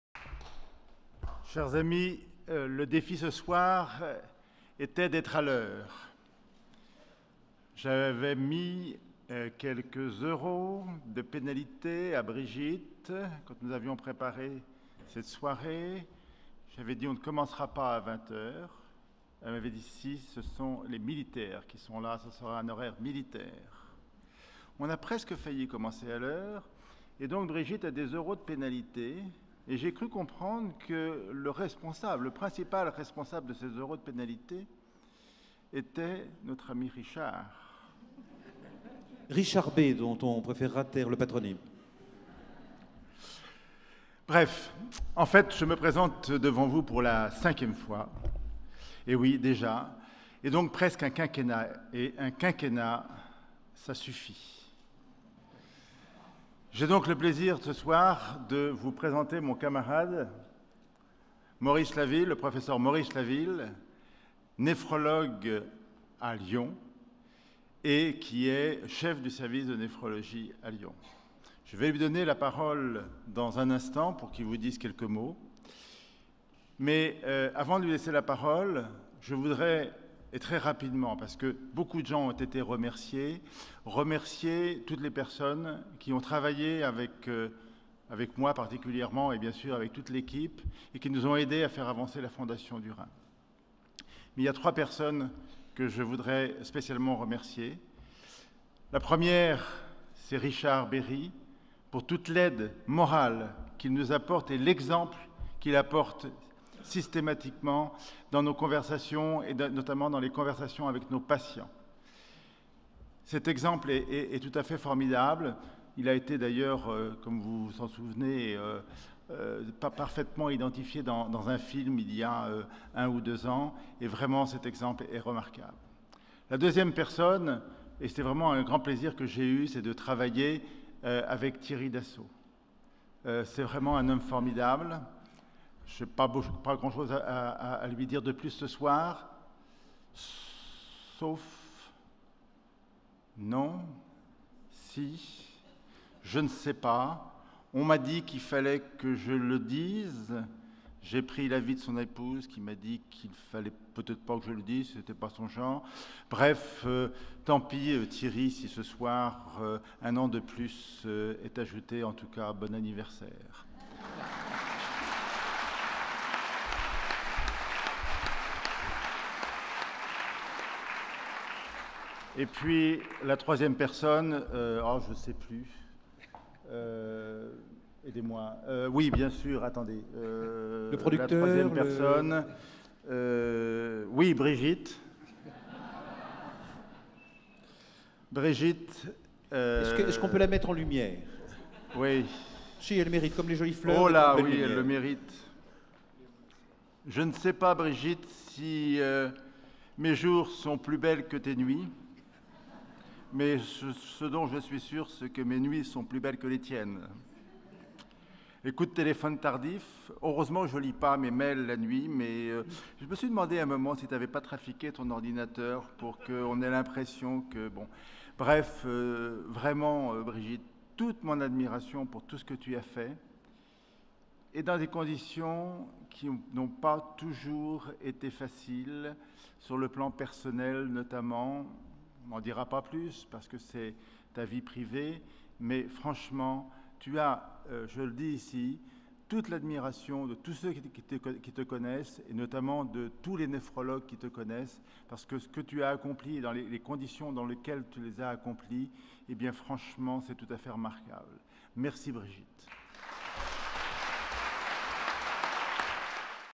Discours du Pr Michel Godin, Président de la Fondation du Rein
Le Pr Michel Godin, Président de la Fondation du Rein, effectue le 26 mars 2015, sa dernière intervention en tant que Président, dans le cadre du Gala de la Fondation du Rein